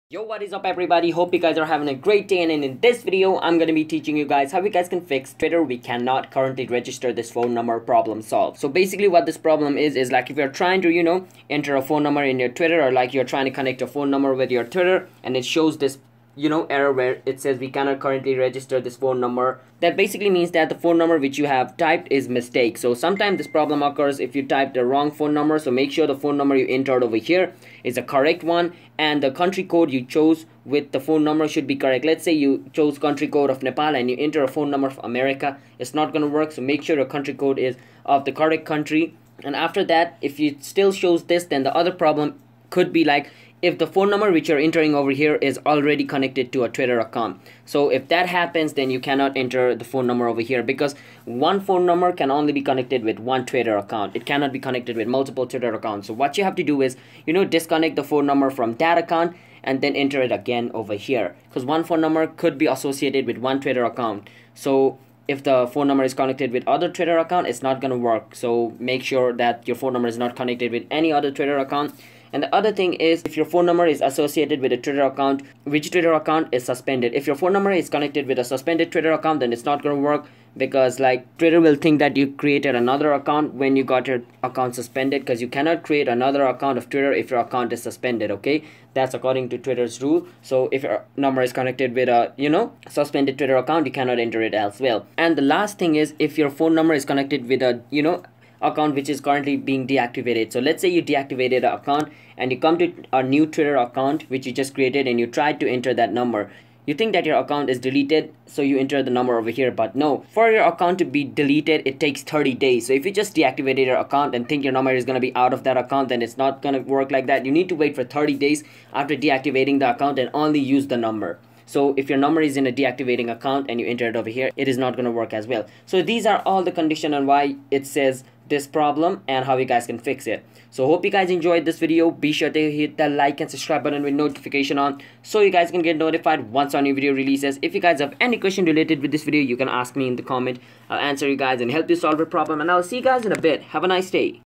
How to Fix Twitter We Cannot Currently Register This Phone Number Error (2021)
In this short and easy tutorial, I’ll be showing you how you can fix the ‘We Cannot Currently register This Phone Number’ issue on Twitter. This video can be very helpful to anyone trying to get rid of this issue quickly.
Twitter is an American microblogging and social networking service on which users post and interact with messages known as "tweets". Registered users can post, like, and retweet tweets, but unregistered users can only read them.
So, to fix the We Cannot Currently register This Phone Number problem on Twitter, follow the steps mentioned below:
1. The first thing you’ll need to do is check if you’ve entered the correct phone number. Entering the incorrect phone number can cause this issue.
2. Also, make sure that the country code you’ve selected is correct as well.
3. If you already have a Twitter account with the same phone number connected to it, then you cannot enter the phone number in the process. You’ll have to disconnect the phone number from the other account to continue.
4. This issue could also occur if the number is associated with the account that has been suspended by Twitter.
5. The other cause of this issue could be if you have the number connected to a deactivated account. You’ll have to wait for 30 days until your deactivated account gets deleted and you can proceed to enter the number.
6. After following all the steps mentioned, you’ll be able to can fix We Cannot Currently register This Phone Number error on Twitter.